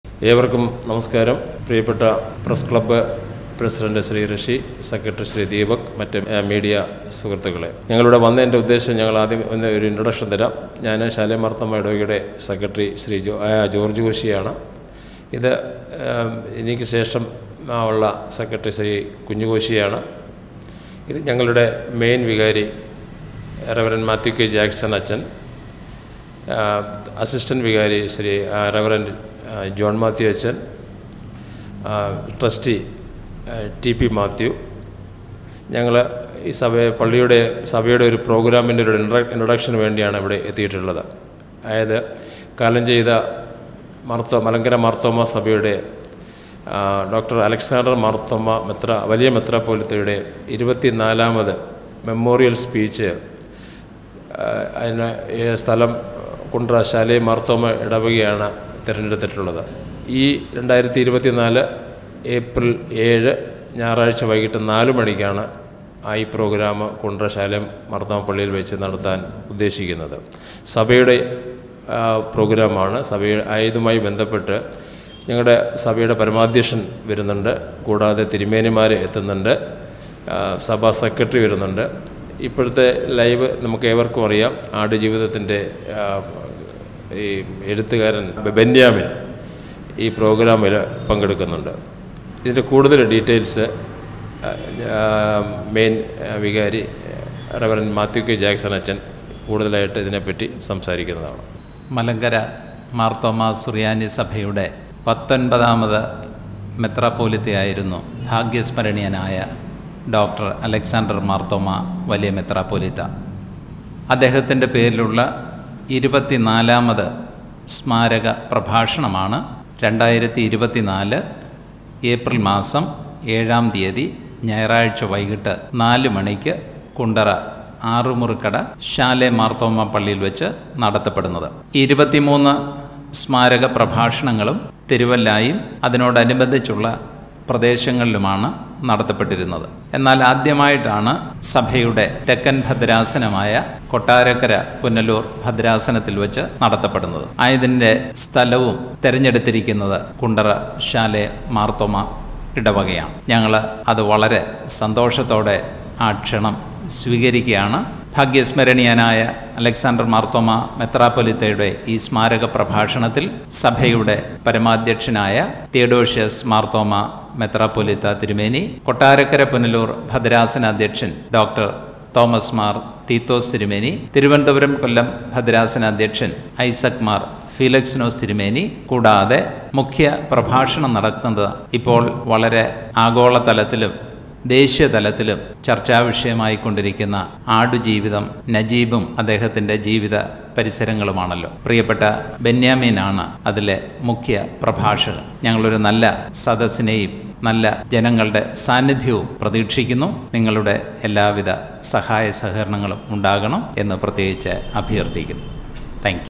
ഡോ. അലക്‌സാണ്ടർ മാർത്തോമ്മാ വലിയ മെത്രാപ്പോലീത്തയുടെ ഇരുപതിനാലാമത് സ്മാരക പ്രഭാഷണം
Press Meet @ Kundara Press Club
മലങ്കര മാർത്തോമ്മാ സുറിയാനി സഭയുടെ കാലം ചെയ്ത ഡോ. അലക്‌സാണ്ടർ മാർത്തോമ്മാ വലിയ മെത്രാപ്പോലീത്തയുടെ ഇരുപതിനാലാമത് സ്മാരക പ്രഭാഷണം 2024 ഏപ്രിൽ 7 (ഞായർ)  വൈകിട്ട് 4 മണിക്ക് കുണ്ടറ ആറുമുറിക്കട  ശാലേം മാർത്തോമാ പള്ളിയിൽ വെച്ച് നടത്തപ്പെടുന്നു.

ഉദ് ഘാടനം HG. മോസ്റ്റ്  റവ.ഡോ. തിയഡോഷ്യസ് മാർത്തോമാ മെത്രാപ്പോലീത്ത നിർവ്വഹിക്കും.  പ്രമുഖ എഴുത്തുകാരൻ ബെന്യാമിൻ (ആട് ജീവിതം ഫെയിം) മുഖ്യ പ്രഭാഷണം നടത്തും.

KUNDARA MEDIA
Facebook | Youtube | Instagram | Website | Threads | Whatsapp
വാർത്തകളും പരസ്യങ്ങളും നൽകാൻ വാട്സാപ്പ് മെസ്സേജ് ചെയ്യുക 👇